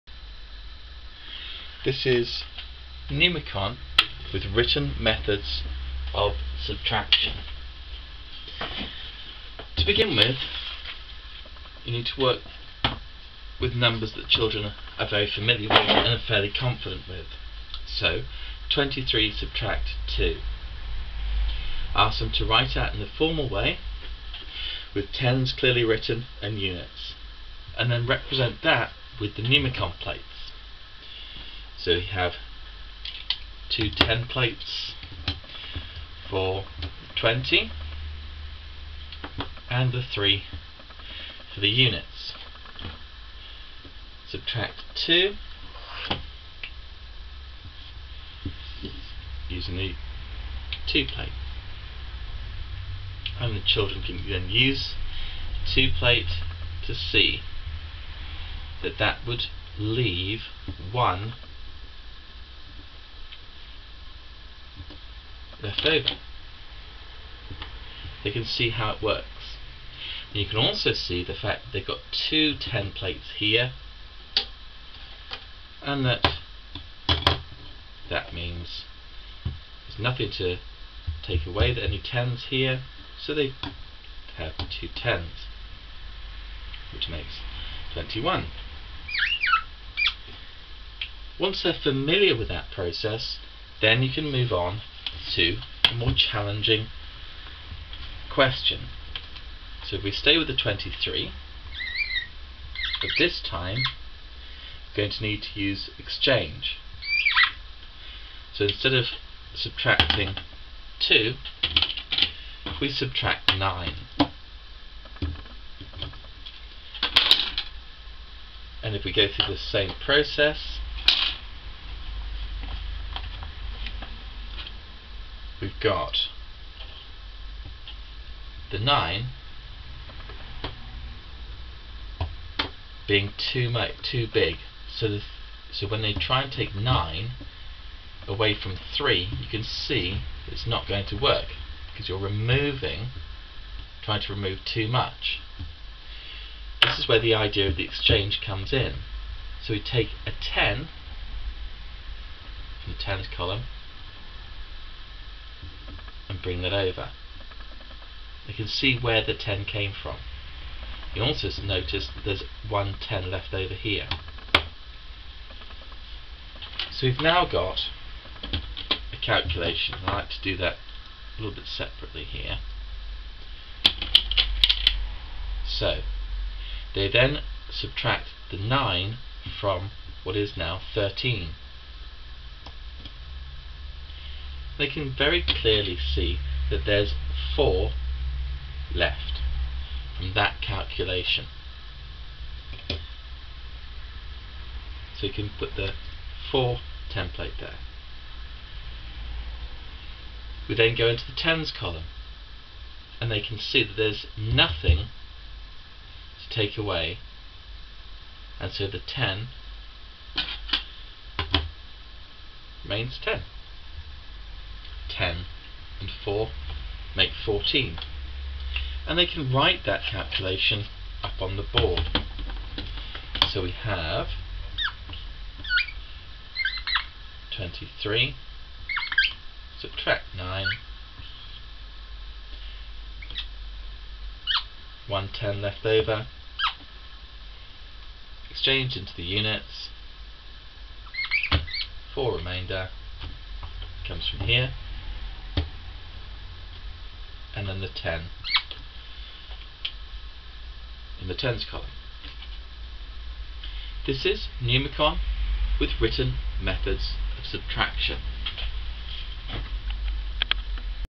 Numicon Written Methods of Subtraction pt2
Numicon helps children understand how to use exchange in written methods of subtraction